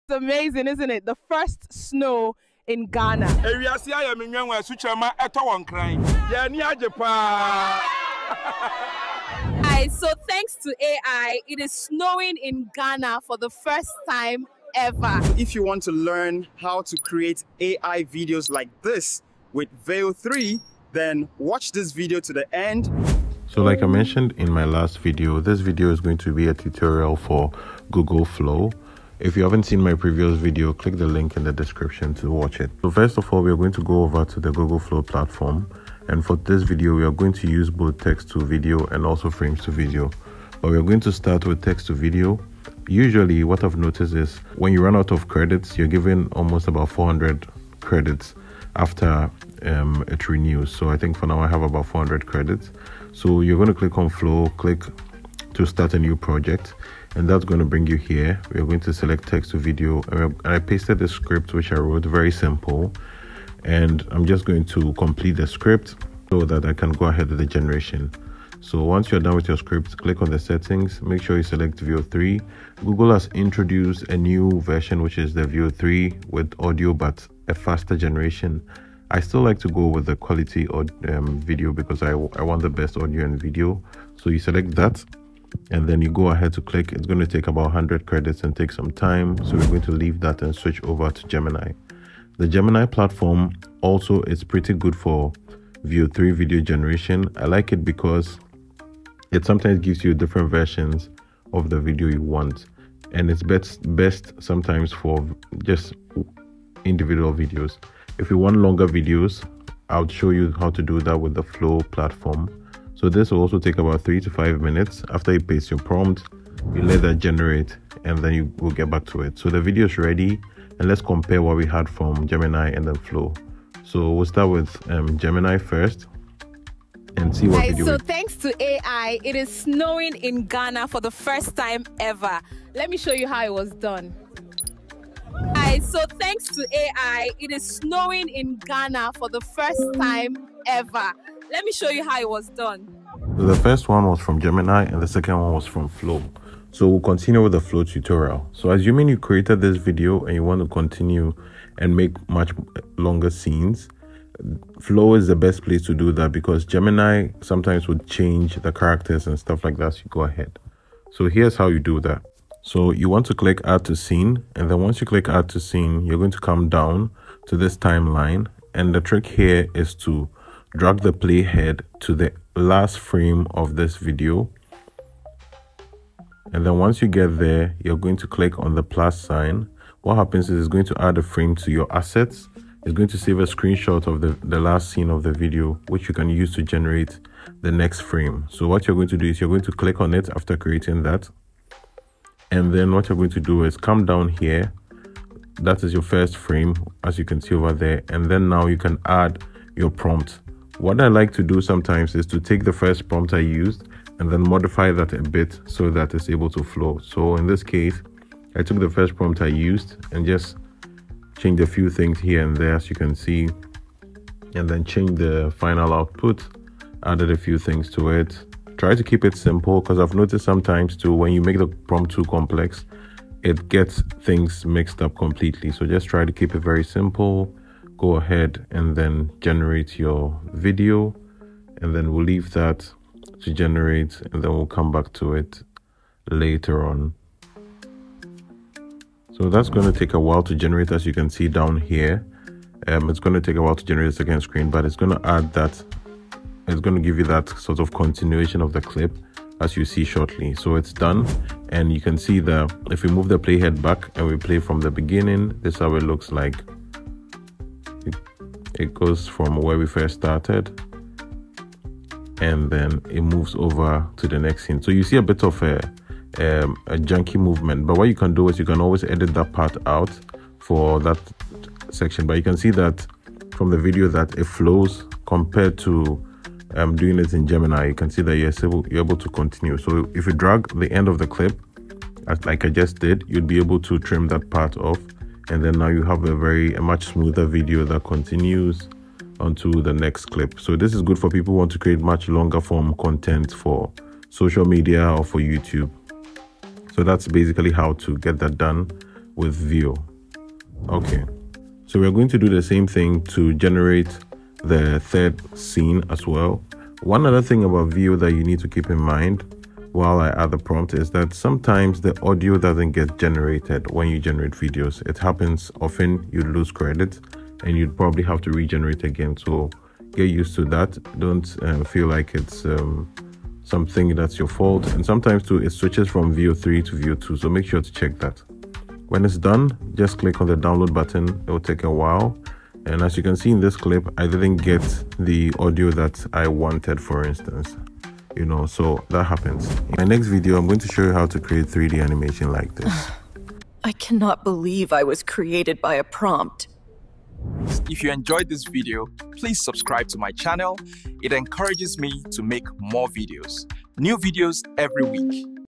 How to make viral long videos with Veo 3 for Free
In this step-by-step guide, learn how to use Google Veo 3 to create viral long videos completely FREE. We break down how to structure your ideas, use Veo’s advanced features, and generate high-quality videos perfect for YouTube, TikTok, or Instagram Reels.

This video is perfect for: Creators looking to grow, Beginners trying Veo 3, AI video content marketers, Anyone wanting viral results without expensive tools.